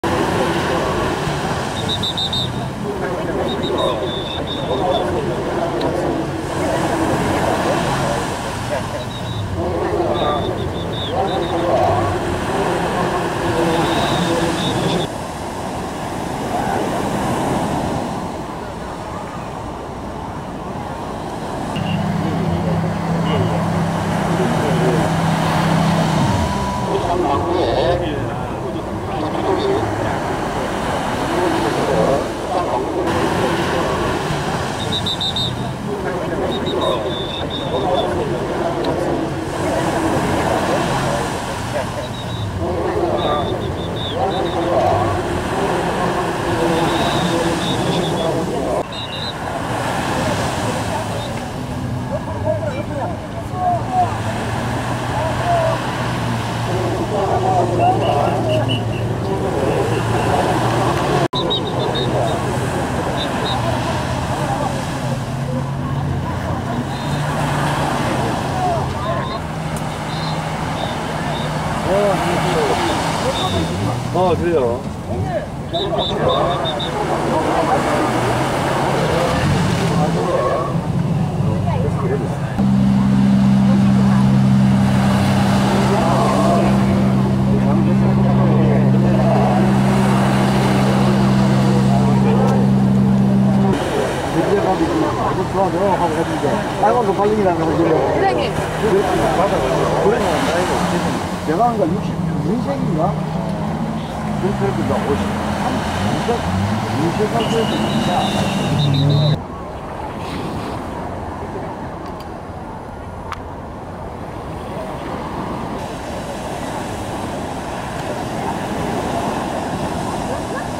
(NAM) 실제 상황이 아닙니다...부산 해운대서 역파도 대비 훈련
10일 오전 부산 해운대 해수욕장에서 해운대여름해양경찰서, 119수상구조대 등이 합동으로 이안류(역파도) 대비 훈련을 하고 있다. 이안류는 해안으로 밀려오다가 갑자기 먼바다 쪽으로 빠르게 되돌아가는 역파도로 지난해 여름 해운대해수욕장에서 62차례 발생했다. /남강호기자